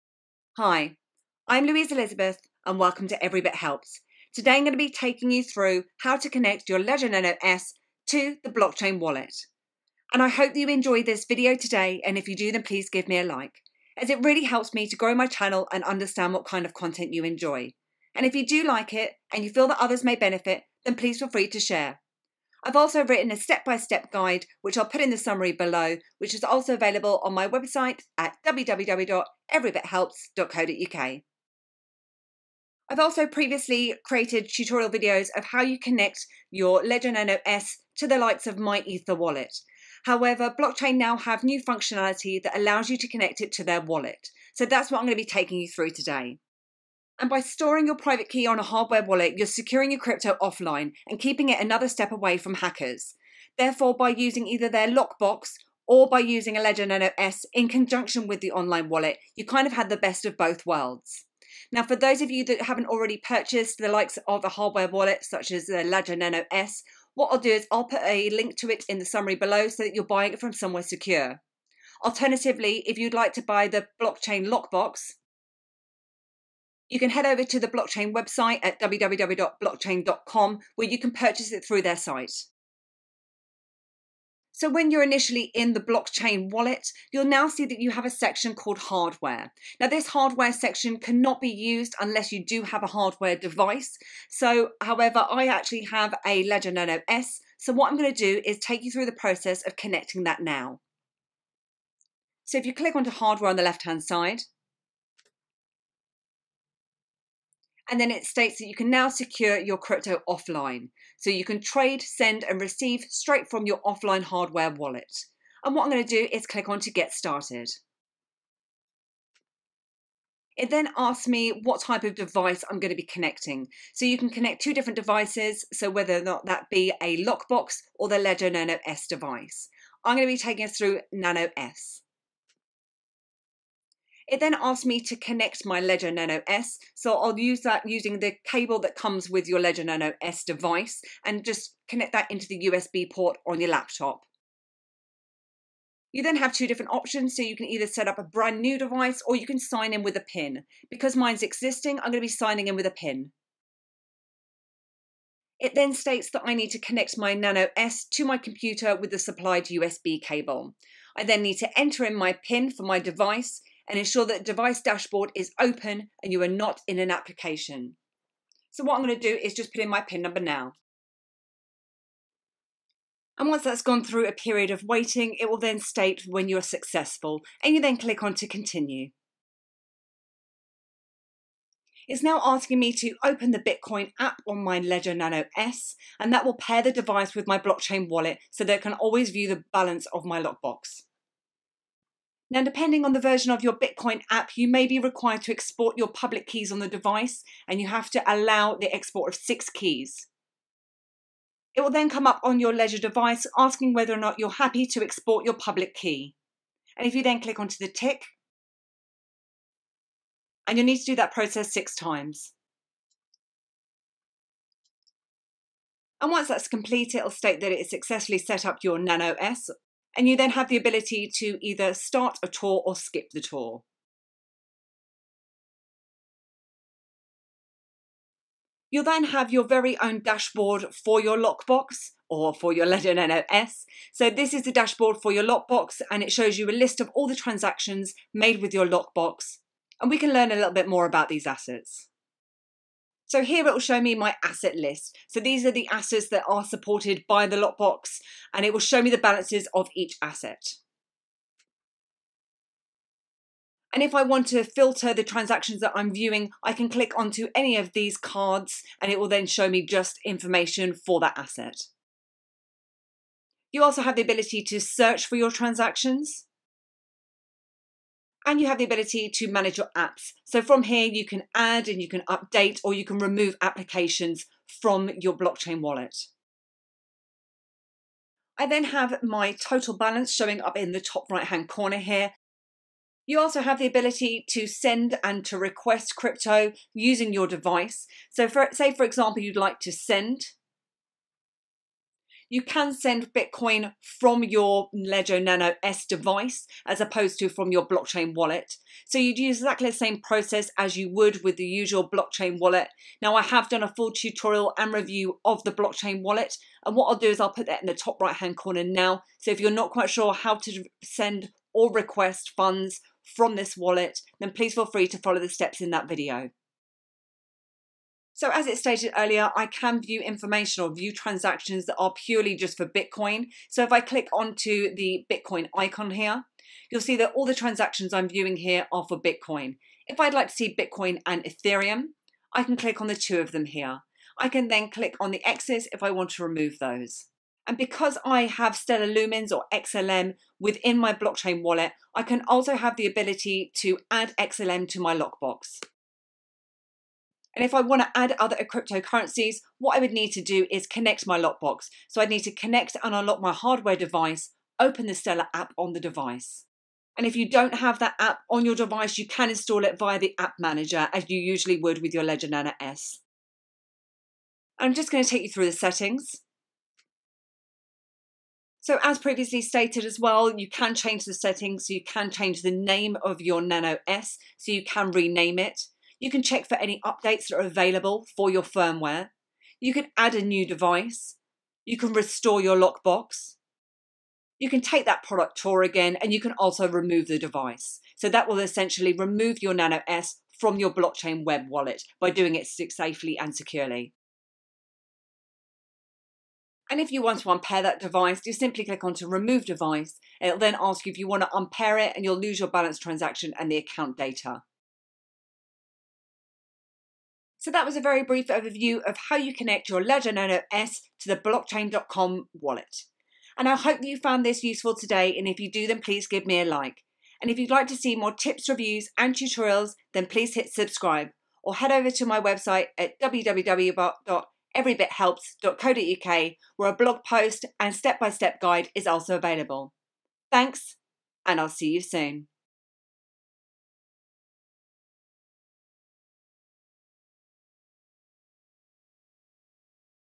Connect Ledger to Blockchain.com Wallet
In this video tutorial, I go through the process of connecting a Ledger Nano S or Blockchain Lockbox to a Blockchain online wallet. This process is very simple and provides you with a more secure way of storing, sending, receiving and exchanging your Bitcoin, Ethereum and other Cryptocurrency. With your assets being stored offline they are 1 step further away from being accessed by potential hackers.

If you don't already own a hardware wallet, you can purchase one from Ledger or use Blockchain's very own Lockbox.